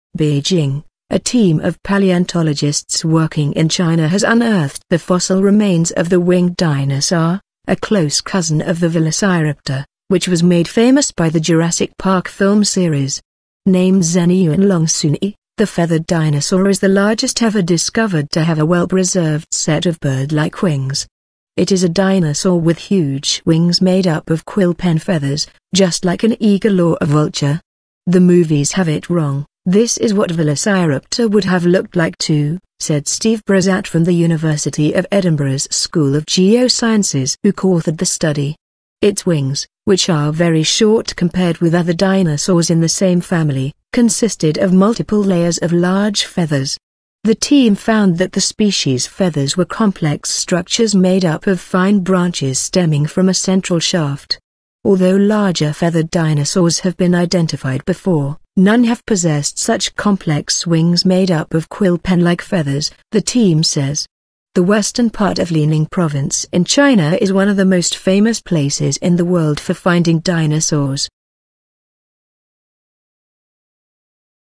Largest Bird Like Dinosaur Unearthed in China
BEIJING:  A team of palaeontologists working in China has unearthed the fossil remains of the winged dinosaur - a close cousin of the Velociraptor - which was made famous by the "Jurassic Park" film series.

Named Zhenyuanlong suni, the feathered dinosaur is the largest ever discovered to have a well-preserved set of bird-like wings.

"It is a dinosaur with huge wings made up of quill pen feathers, just like an eagle or a vulture. The movies have it wrong - this is what Velociraptor would have looked like too," said Steve Brusatte from the University of Edinburgh's School of Geosciences who co-authored the study.

Its wings - which are very short compared with other dinosaurs in the same family - consisted of multiple layers of large feathers.

The team found that the species' feathers were complex structures made up of fine branches stemming from a central shaft.

Although larger feathered dinosaurs have been identified before, none have possessed such complex wings made up of quill pen-like feathers, the team says.

The western part of Liaoning Province in China is one of the most famous places in the world for finding dinosaurs.